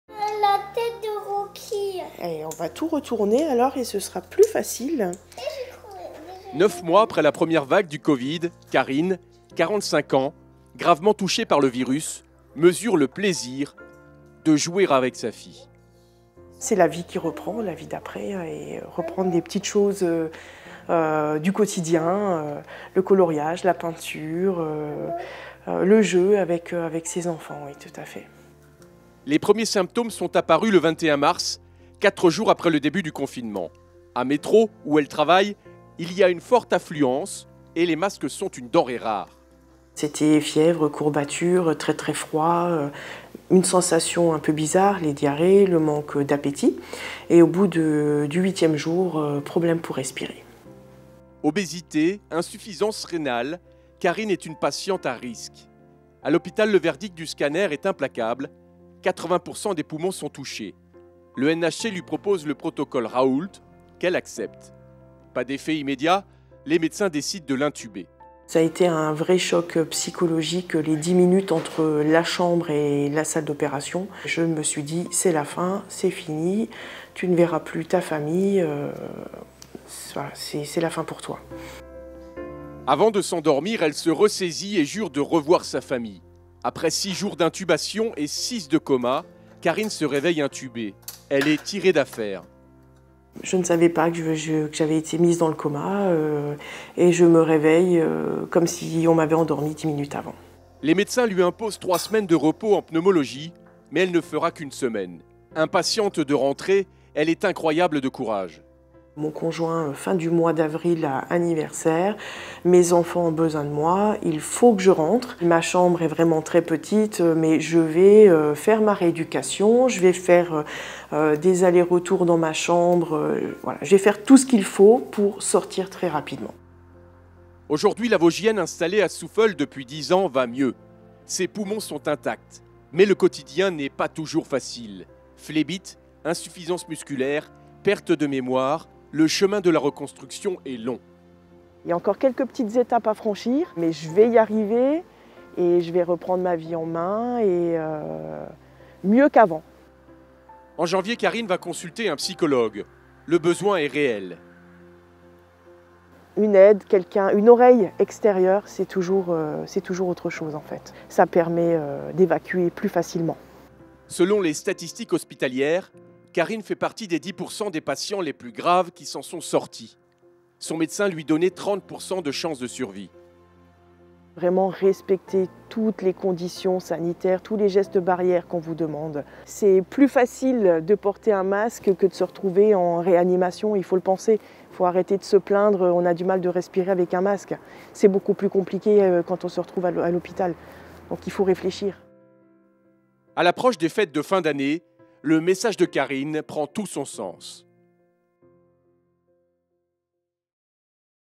187. Une victime de la COVID-19 témoigne - Souffel PLAY
Le Coronavirus a aussi touché les Souffelweyersheimois. Une habitante gravement atteinte et rescapée témoigne pour nous.
Respectons les gestes barrière !